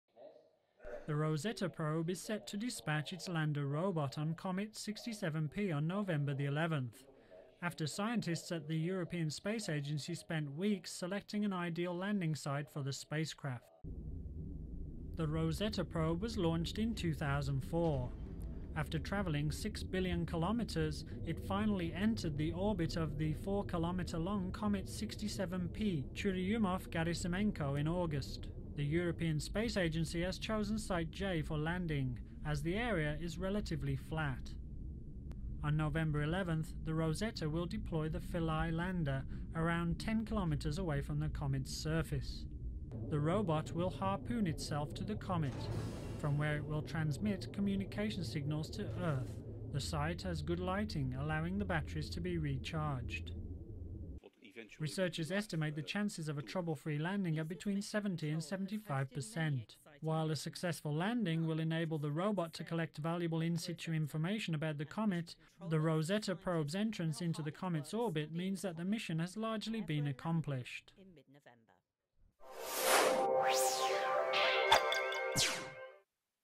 Rosetta spacecraft ready to land on Comet 67P/Churyumov–Gerasimenko
The Rosetta probe is set to dispatch its lander robot on Comet 67P on November 11, after scientists at the European Space Agency spent weeks selecting an ideal landing site for the spacecraft.

After travelling six billion kilometres, the Rosetta probe, which was launched in 2004, finally entered the orbit of the four kilometre-long Comet 67P/Churyumov-Gerasimenko in August.

The European Science Agency has chosen site J for landing, as the area is relatively flat. Scientists initially selected five potential landing sites. They also selected a back-up site C, which is located on Comet 67P’s larger lobe.

On November 11, the Rosetta will deploy its Philae lander about 10 kilometres away from the comet. The robot will harpoon itself to the comet from where it will transmit communications back to Earth. The process of detachment and landing will take about seven hours.

The site also has good lighting, which will allow the craft’s solar batteries to be recharged, the BBC reported. Both sites J and C are now mapped, which will hopefully provide more details and ensure a smooth landing.

Researchers estimate the chances of a trouble-free landing at between 70 and 75 percent. It is still a tortuous endeavour, as site J also has cliffs and boulders. The gravitational pull is also significantly lower than Earth, making a soft-landing extremely  difficult.

While a successful landing will enable the robot to collect valuable in-situ information about the comet, the Rosetta probe’s entrance into the comet’s orbit means that the mission has largely been accomplished.